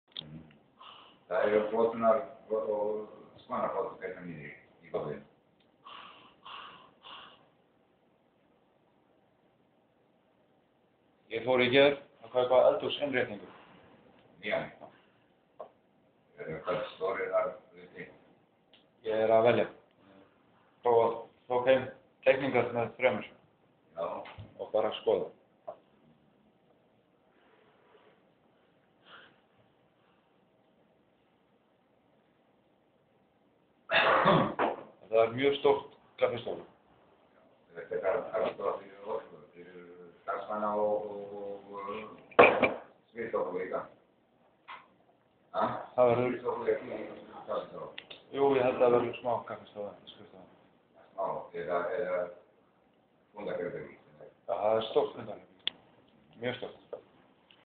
agustino at coffe break
my brother agustino:) at coffe break,he just woke up and came to coffe break and start to speak some weird language;p